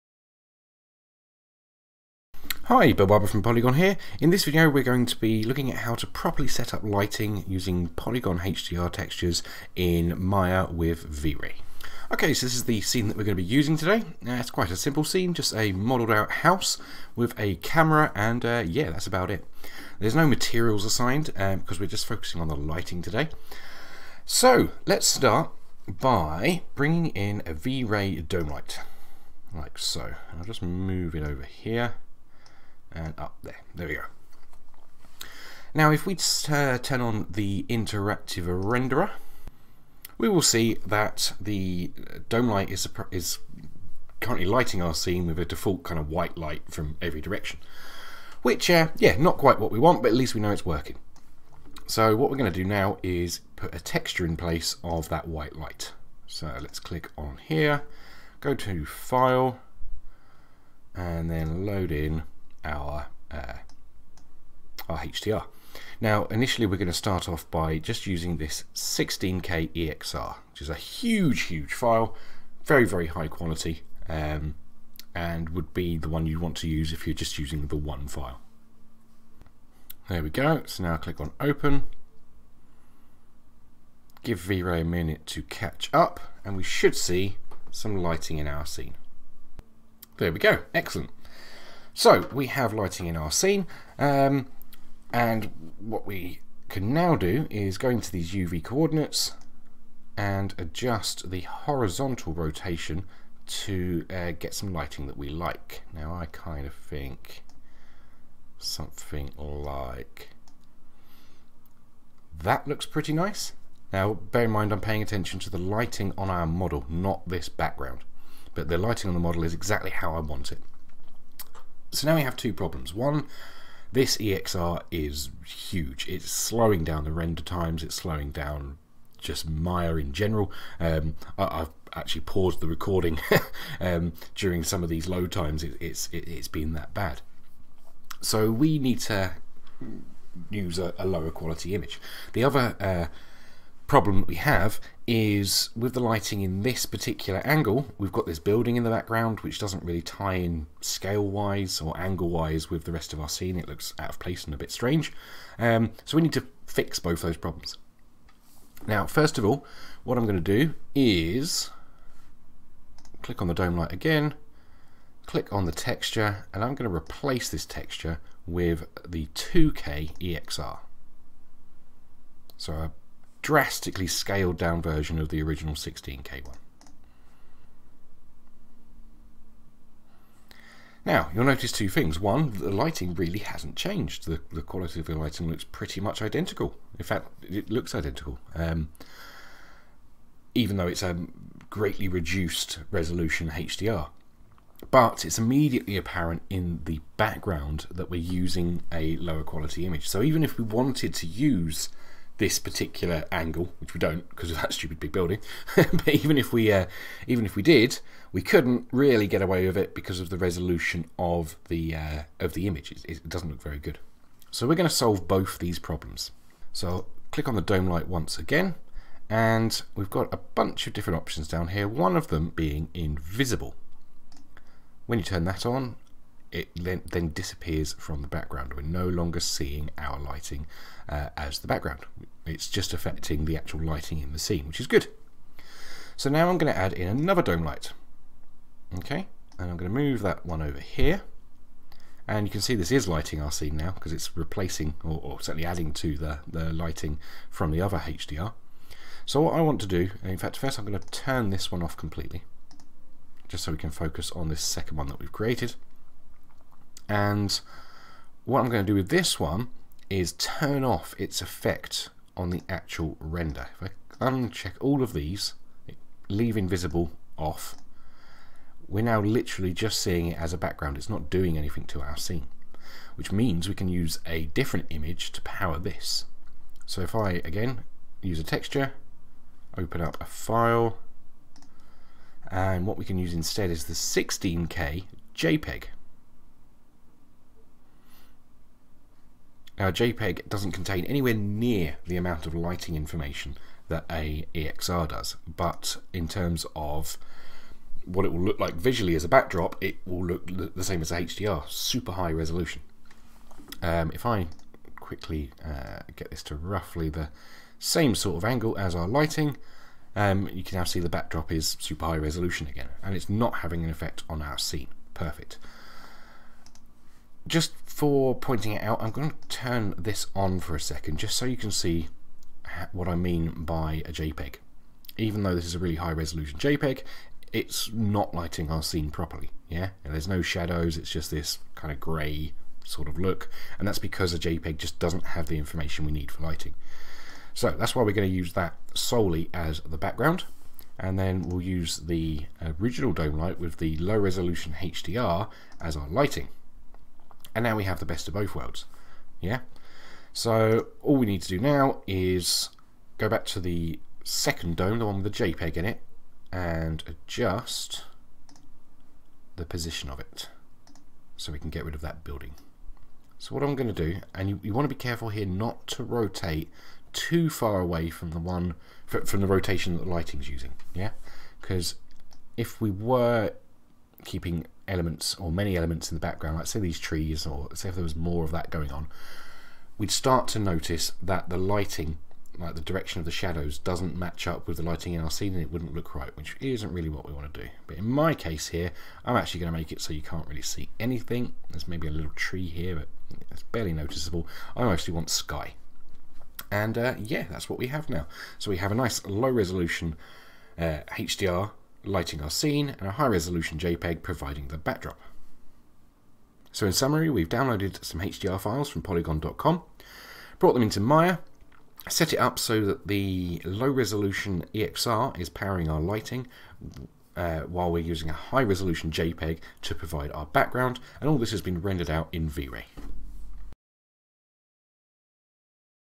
Using Poliigon HDRs to light a scene in Maya with V-Ray
In this tutorial we will be exploring how to light a scene using a low resolution .exr with a high resolution .jpg as the background to limit resource usage and increase system responsiveness without compromising the quality of our finished render.